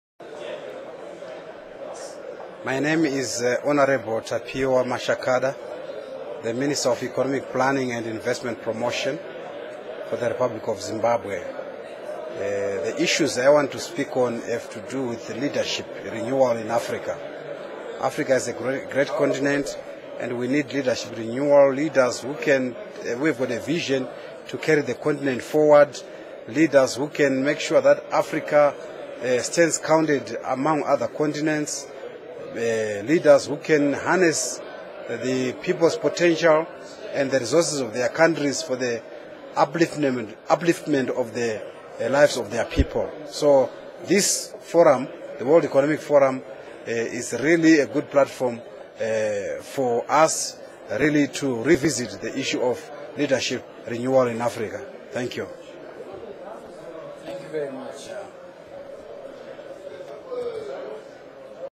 Africa 2012- Tapiwa Mashakada
Tapiwa Mashakada, Minister of Economic Planning and Investment Promotion of Zimbabwe, responds to the One Year One Change campaign at the World Economic Forum on Africa, Addis Ababa, Ethiopia, 10-11 May 2012.